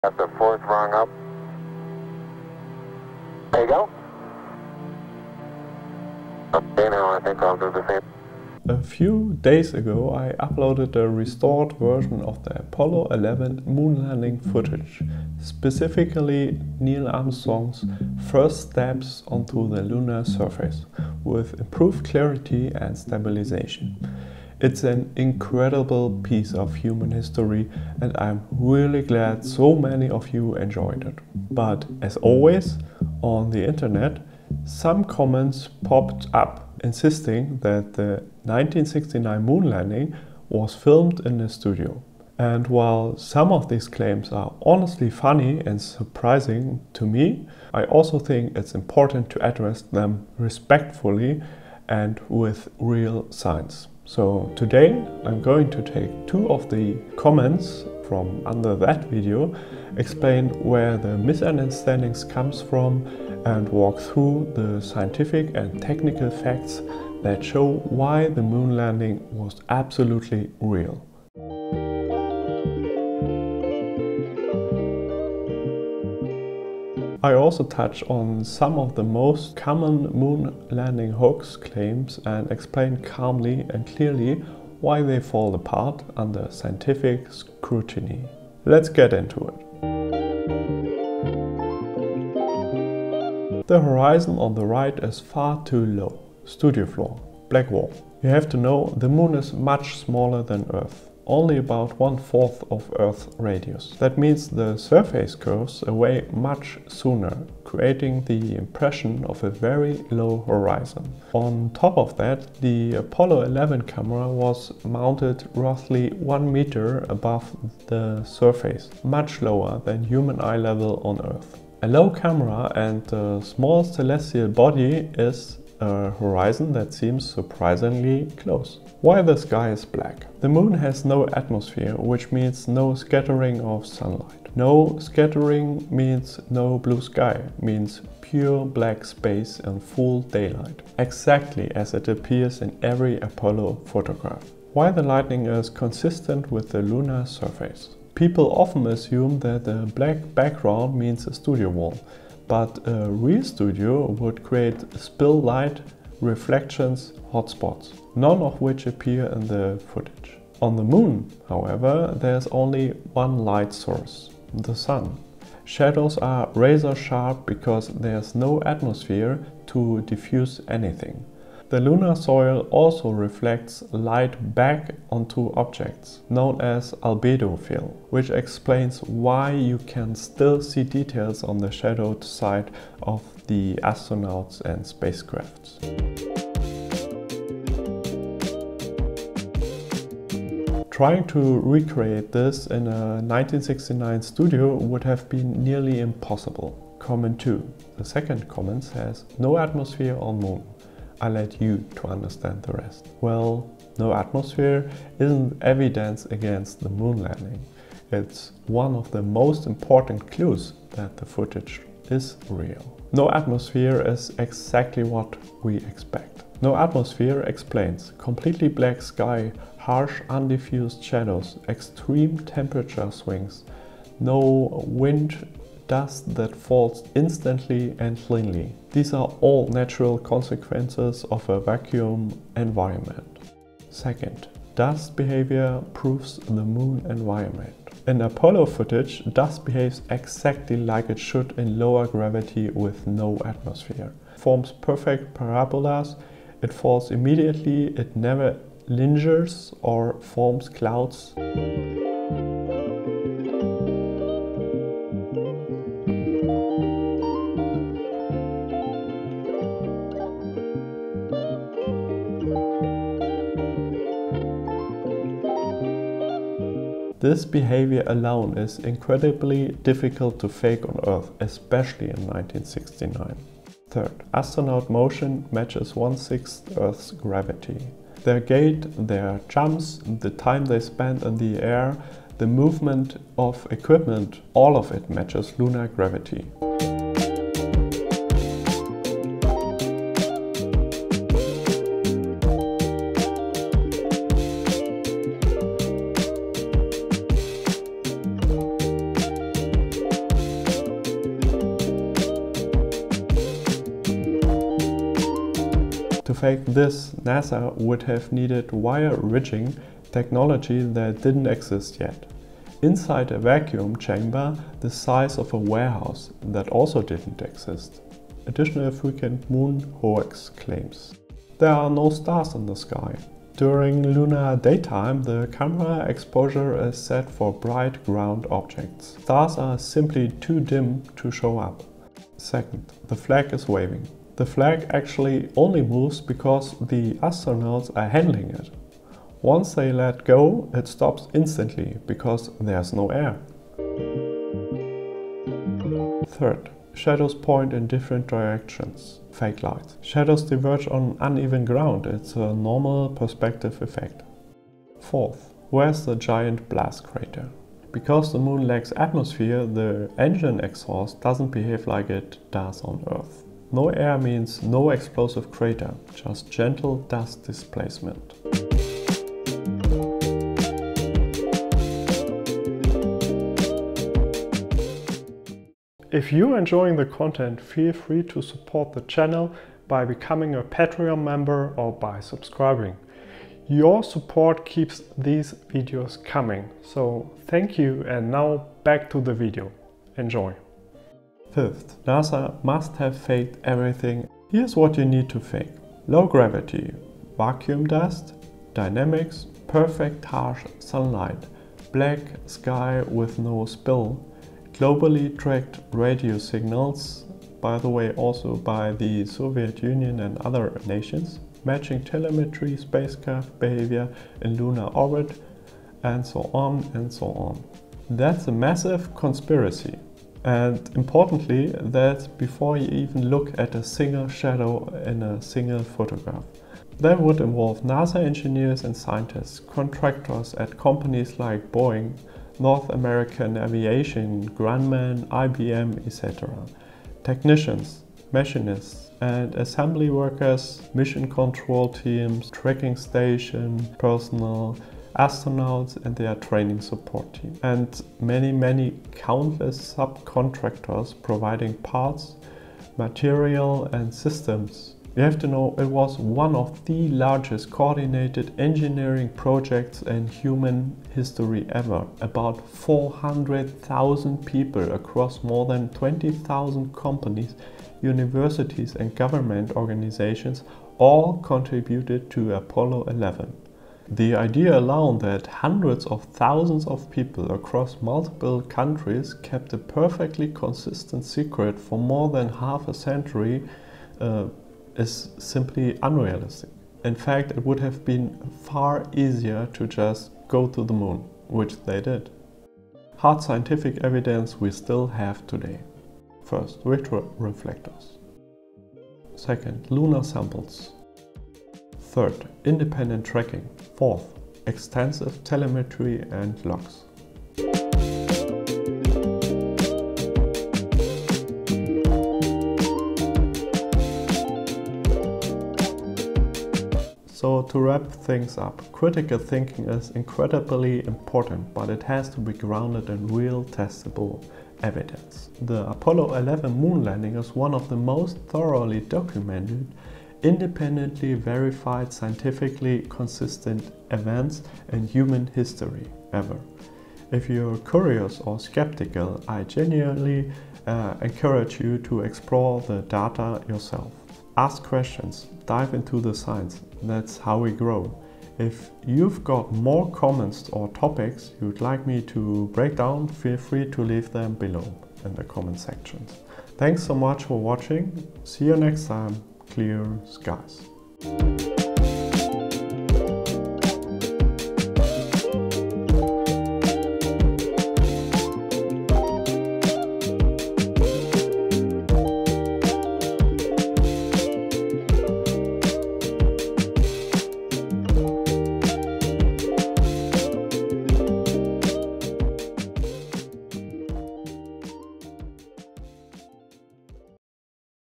Is the Moon Landing Fake? Let’s Look at the Science
Did Apollo 11 land on the Moon — or was it filmed in a studio?
In this video, I respond to two real comments under my restored Apollo 11 footage and explain, with real physics and engineering, why the Moon landing was absolutely real.

0:00 Intro
1:30 YouTube Comment: Why the lunar horizon looks “too low”
2:09 Why the sky is black
2:24 Why shadows look sharp
3:16 YouTube Comment: No atmosphere
3:53 How dust behaves in a vacuum
4:33 Why the astronauts’ movements match 1/6 gravity
5:18 Additional Moon Hoax Claims (No Stars, Flag, Shadow Directions, Crater)
6:31 Claim: NASA Faked Everything
8:12 Evidence like retroreflectors, moon rocks, and independent tracking
8:37 Outro & Own Thoughts

Credit (@ 4:11)